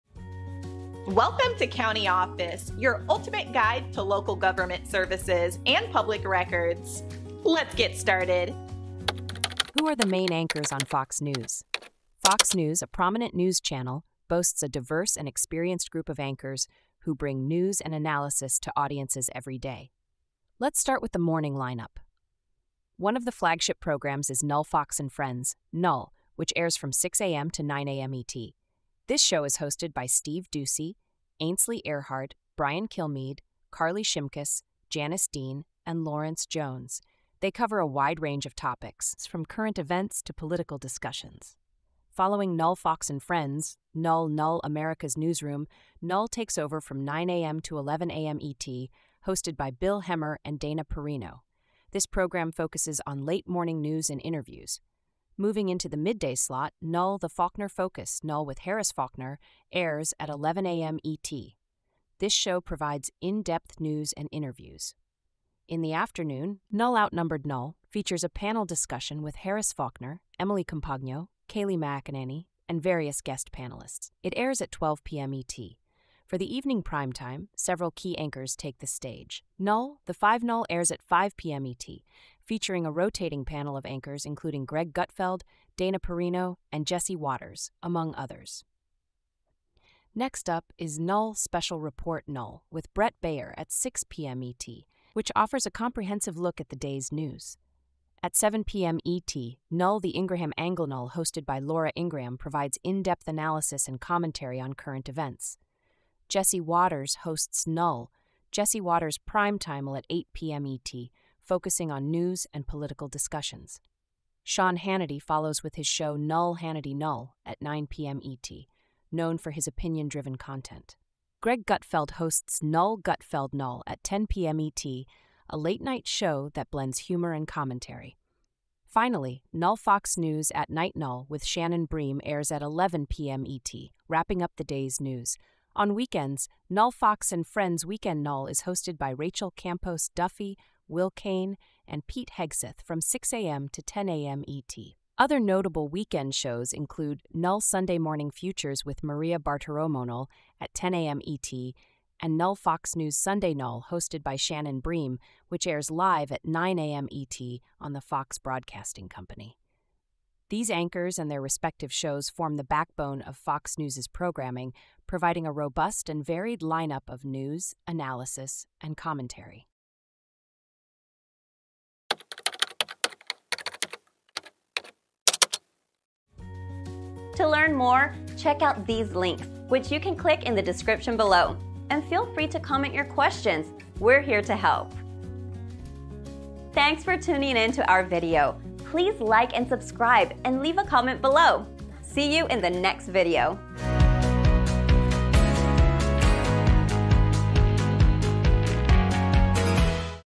Who Are the Main Anchors on Fox News? | CountyOffice.org
Who Are the Main Anchors on Fox News? In this engaging video, we explore the dynamic lineup of anchors on one of America's leading news networks. Each anchor brings a unique perspective and style to their respective shows, shaping the way news is presented and discussed. From early morning broadcasts to late-night commentary, these anchors cover a wide array of topics, including current events, politics, and in-depth interviews.

We will take you through the daily schedule of Fox News, highlighting flagship programs such as "Fox & Friends," "America's Newsroom," and "The Ingraham Angle." You'll get to know the anchors who deliver both news and analysis, providing viewers with a comprehensive understanding of the day’s events. Whether you're interested in political discussions or lighthearted commentary, there's something for everyone in this lineup.

Join us as we delve into the world of Fox News anchors, their shows, and how they contribute to the channel's overall programming.